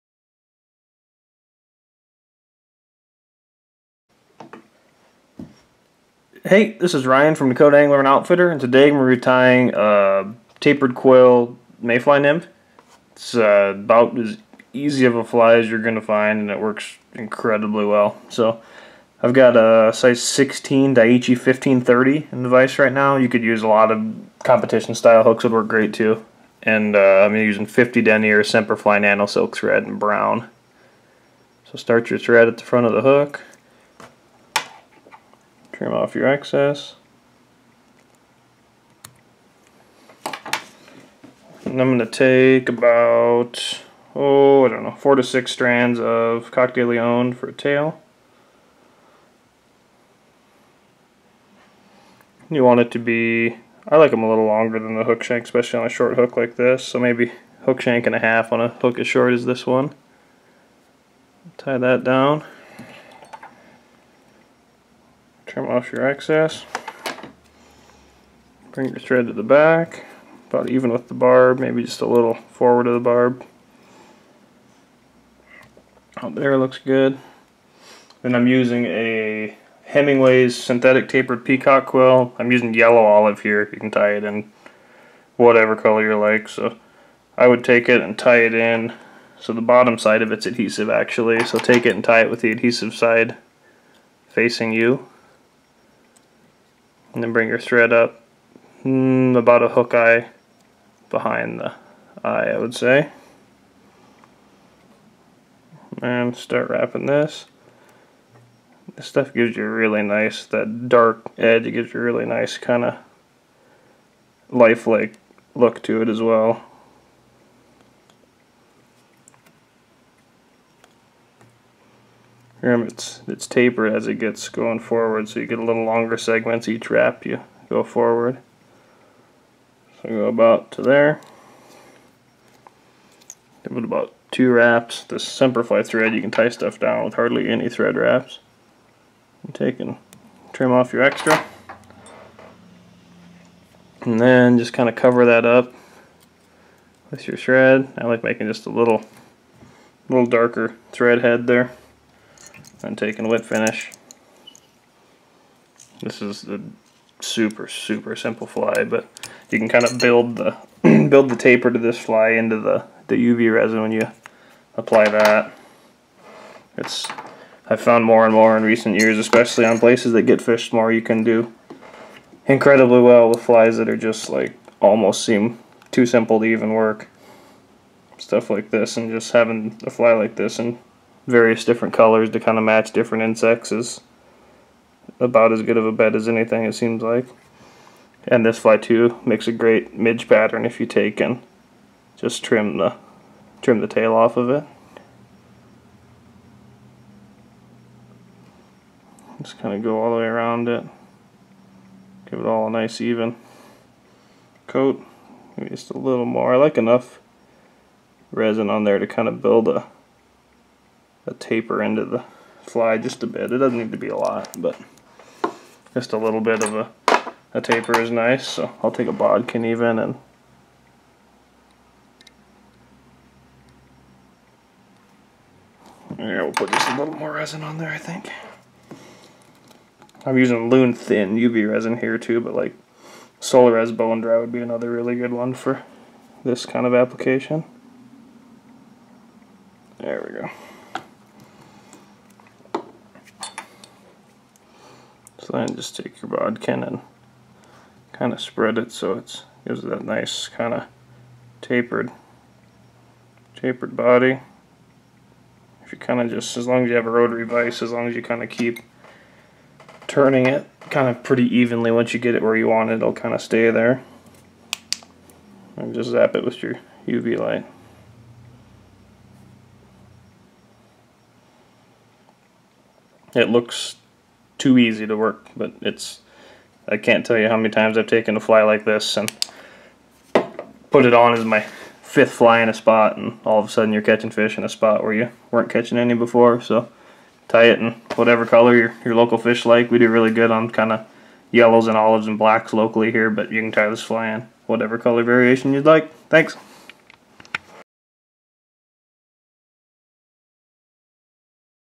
Fly Tying with Ryan - Killer Quill Mayfly
We call this fly the Killer Quill, and for good reason - when fish aren't eating anything else, they dig skinny mayfly patterns. Super simple & effective pattern, plus it's a quick tie. If you have picky fish eating little mayfly nymphs, this is a pattern to definitely have in your box. Brown, yellow, olive, and black are all good bets, but various other colors can work just as well. This fly might not look like much, but it's a winner!